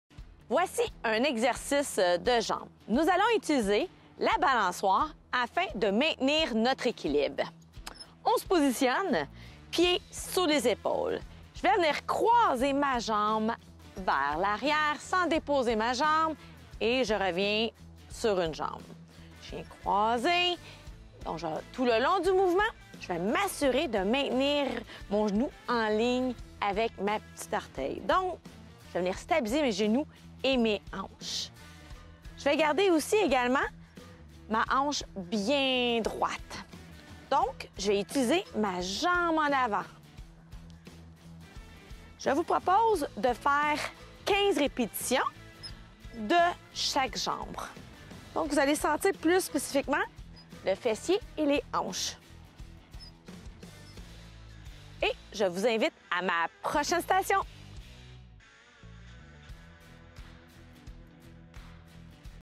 Exercice 3 au parc de Dorion-Gardens
Le Centre Multisports vous propose des entraînements élaborés par un kinésiologue à effectuer dans ces parcs, dans le cadre de l'initiative Saines habitudes de vie. (exercice 3 de 8)